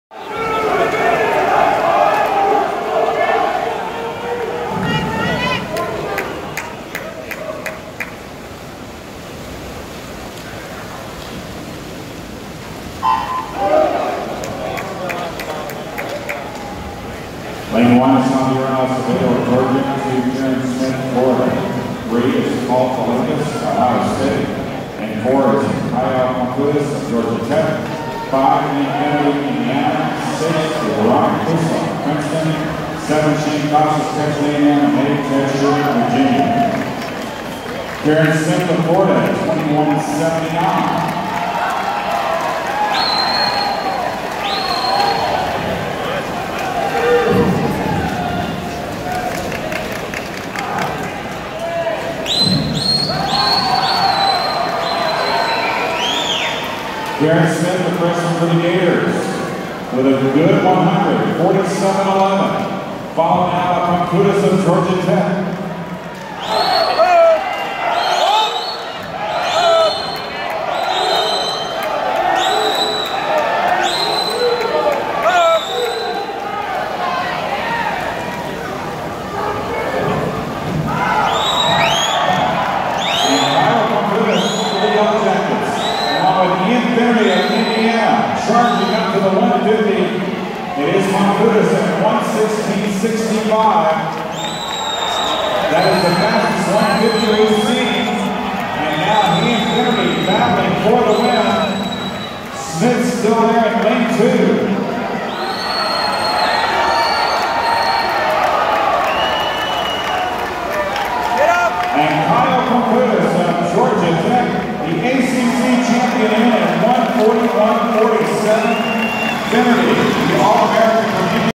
2019 NCAA men's 200 IM heat 5/7
1. Caio Pumputis, Georgia Tech, 1:41.47
2. Ian Finnerty, Indiana, 1:41.80
3. Kieran Smith, Florida, 1:42.26
4. Shaine Casas, Texas A&M, 1:42.29
5. Javier Acevedo, Georgia, 1:43.00
6. Raunak Khosla, Princeton, 1:43.48
7. Paul DeLakis, Ohio State, 1:43.61
8. Ted Schubert, Virginia, 1:44.79